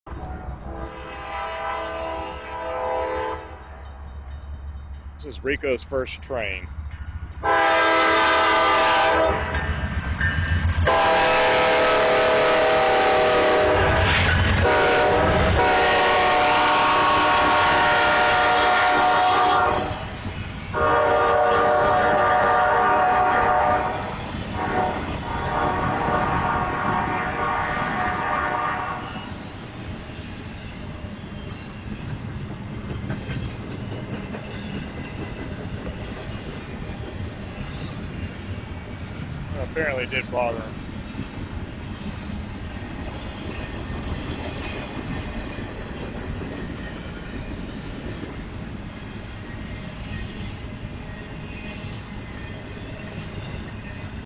Rico first train HSV DP.3GP
Zeke, Sam, and Rico in the Huntsville AL dog park.